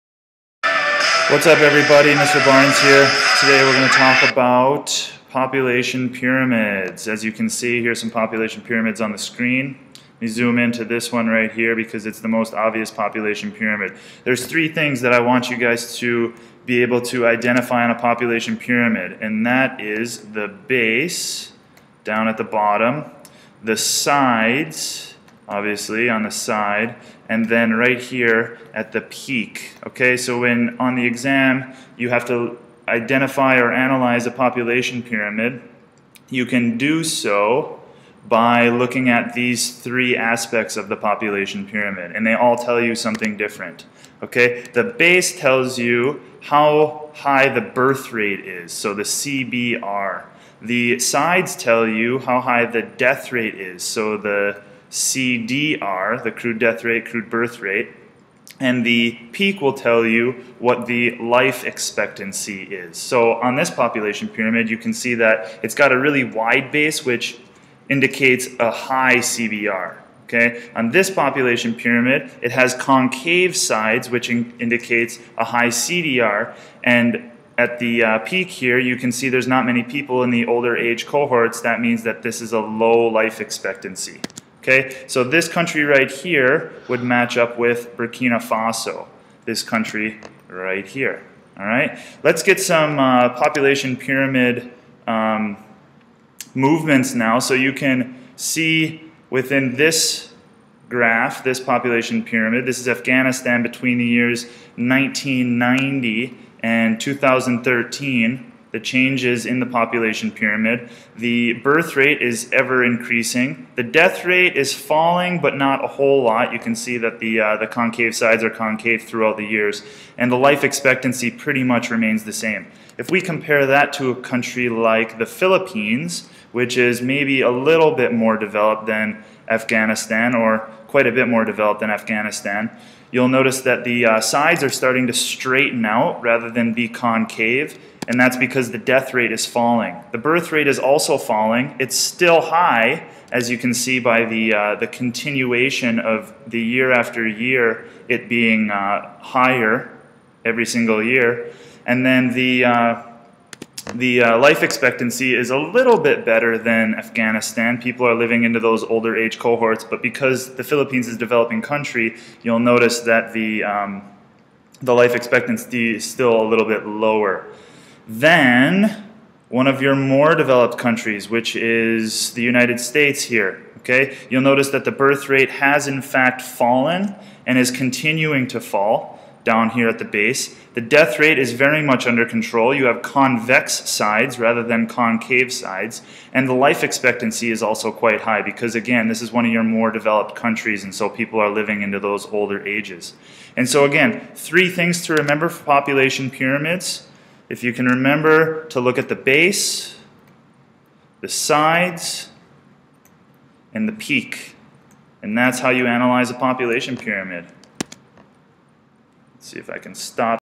Population 1b. Population pyramids
In this video I describe what to look for when analyzing population pyramids.